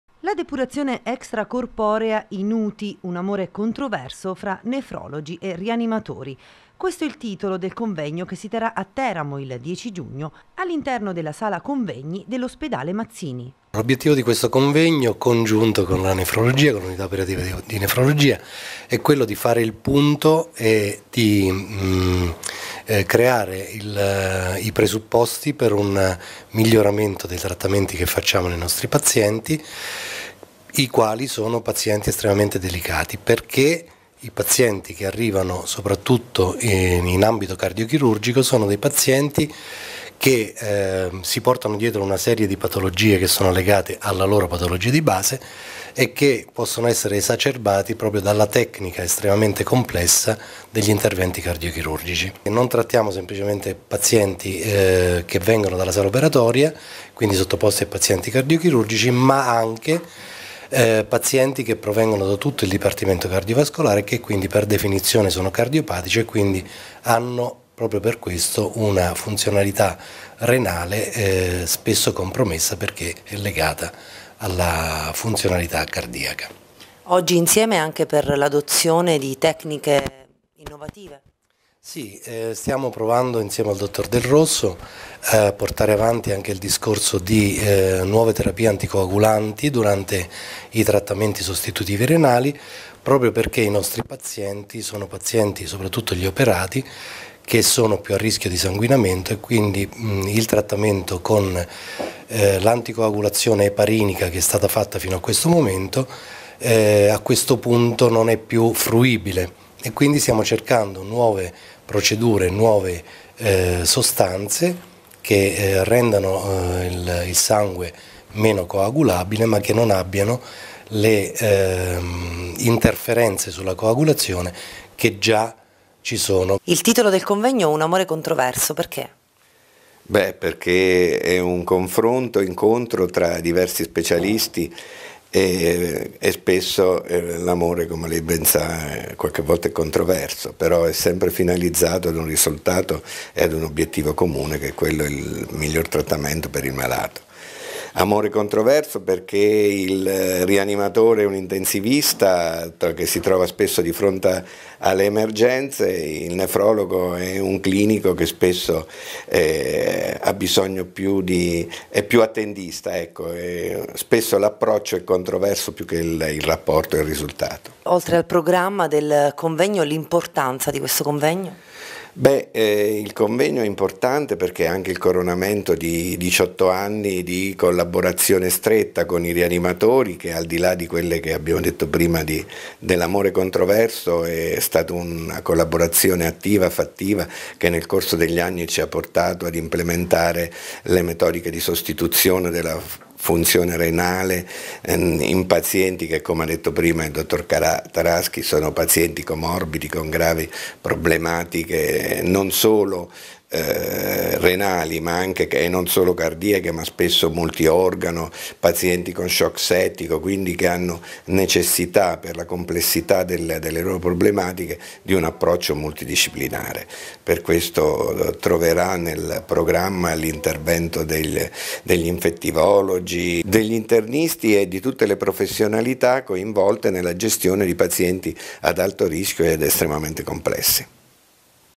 A Teramo il convegno tra Nefrologi e rianimatori
“La depurazione extra corporea in uti. Un amore controverso fra nefrolgi e rianimatori”. Questo è il titolo del convegno che si terra a Teramo il 10 giugno presso l’ospedale Mazzini